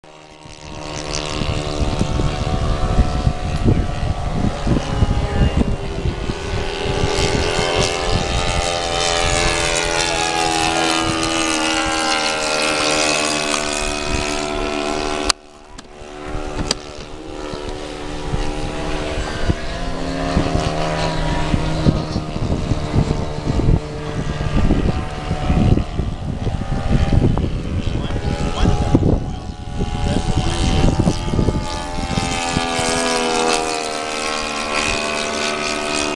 Model Airplanes at a local flying field.
This field is mainly electric with a few glow or gas powered models.  Just a short video of a few flying around.